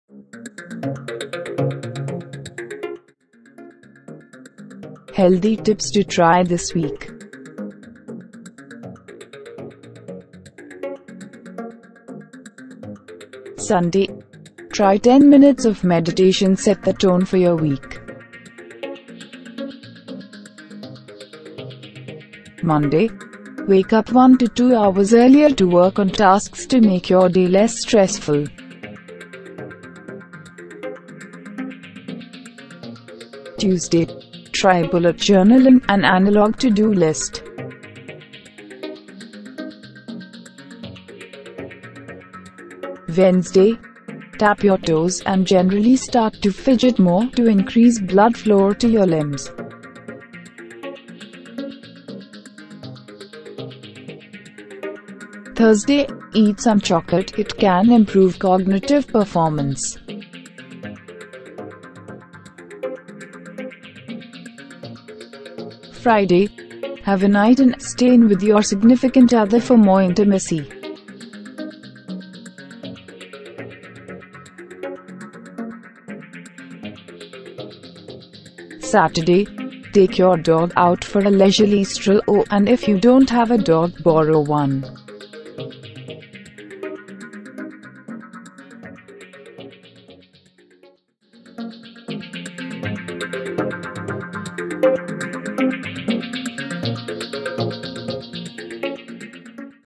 Healthy Tips To Try This Week - Nutshell School
Healthy Tips To Try This Week | Regular Habit, Lifestyle & Fitness Routine

Healthy Tips To Try This Week

Sunday – try ten minutes of meditation set the tone for your week.
Monday- wake up 1-2 hours earlier to work on tasks to make your day less stressful.
Tuesday- try bullet journaling, an analog to-do list.
Wednesday- tap your toes ( and generally start to fidget more) to increase blood flow to your limbs.
Thursday- eat some chocolate: it can improve cognitive performance.
Friday- have a night in. stay in with your significant other for more intimacy.
Saturday- take your dog out for a leisurely stroll. oh, and if you don?t have a dog, borrow one.

To ensure great health, you need a combination of conventional and alternative practices. Illness is not cured with one pill. Stress is not relieved with one massage, and health cannot be restored without looking at the big picture.

*35 Simple Health Tips
*Develop relationships with health care practitioners you trust.
*Eat fruits and veggies.
*Rest when you are sick or tired.
*Be as kind to yourself as you are to everyone else.
*Do something that scares you.
*Trust your body.
*Floss.
*Maintain a healthy weight.
*Drink water.
*Quit smoking.
*Ask for help.
*Offer help.
*Meditate
*Visit the dentist.
*Walk barefoot.
*Slow down.
*Breathe Deeply.
*Sit Still.
*Spend time alone.
*Spend time with friends.
*Eat Colors not Calories.
*Get regular screenings.
*Sleep well.
*Drink Tea.
*Turn your phone off in the car.
*Try chia seeds.
*Stop making excuses.
*Practice Yoga and Save the World
*Move every day.
* Read 10 Ways to Improve Your Health.
*Cook your own food.
*Be Grateful.

Most of these tips are common sense, but without the time, space and attention, our personal health care sometimes moves to the bottom of the list in terms of priority. It is your responsibility to be healthy. It is your obligation to take care of you. Doctors can help, but your body will tell you what you need. Take the time to listen.

What do you think of these health tips? Anything you would add? Anything you do already that makes you feel great?

Nutshell School Tags